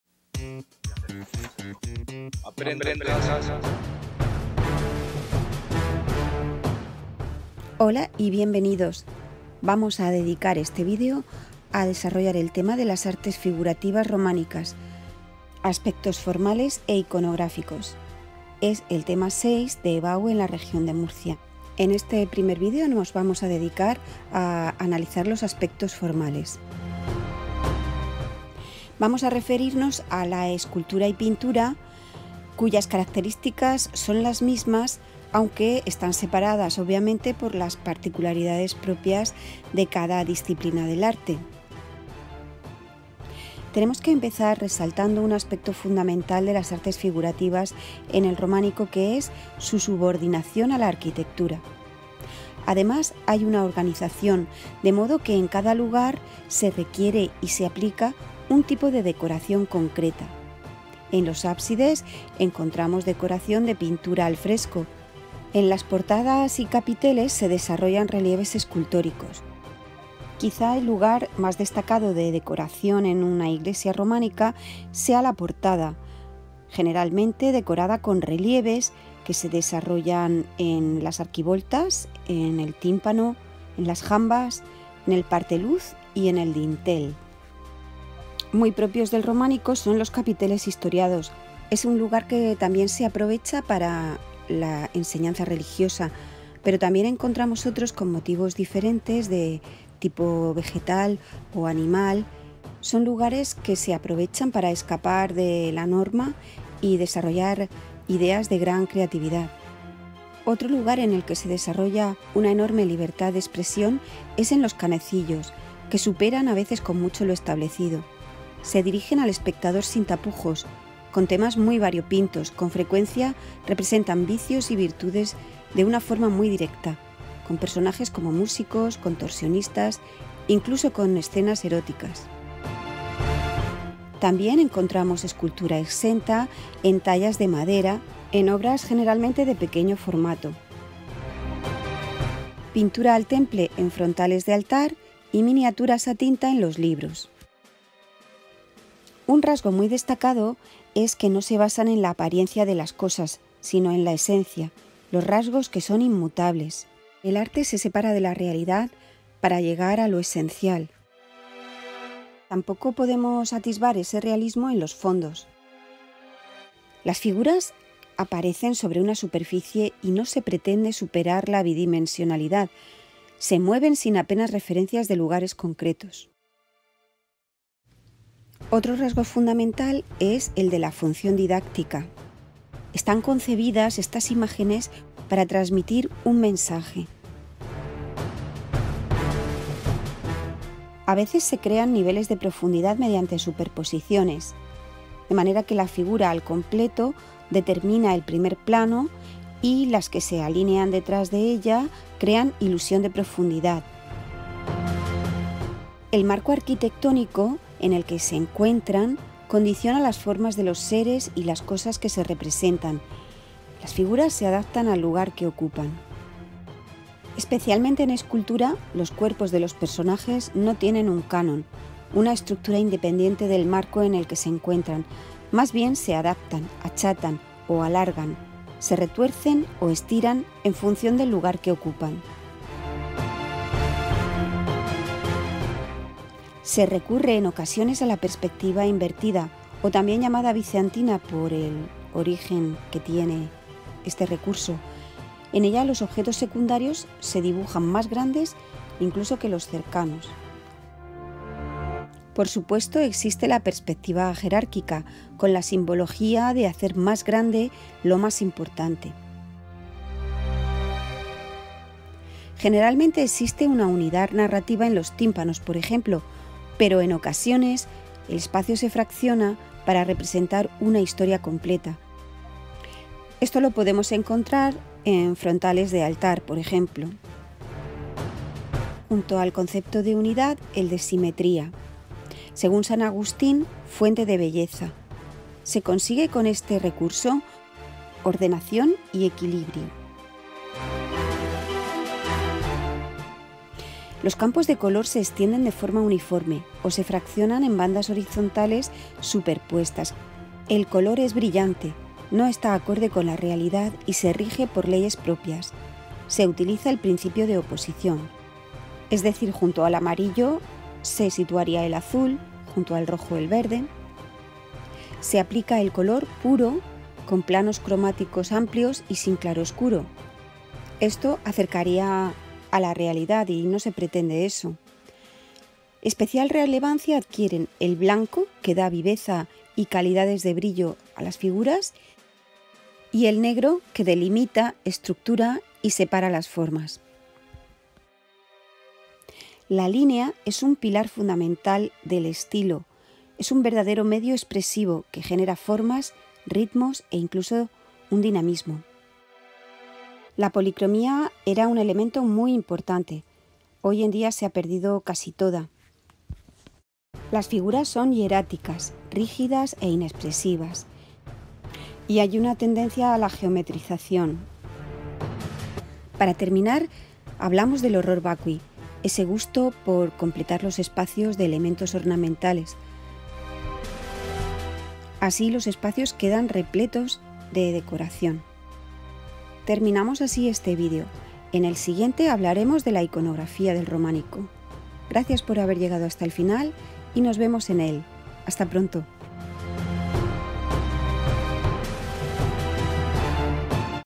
LAS ARTES FIGURATIVAS EN EL ROMÁNICO: ASPECTOS FORMALES E ICONOGRÁFICOS. 1ª PARTE: ASPECTOS FORMALES
En ese vídeo se trata la parte correspondiente a los aspectos formales de las are figurativas en el románico. Es el primero de los que abordan el tema 6 de EBAU.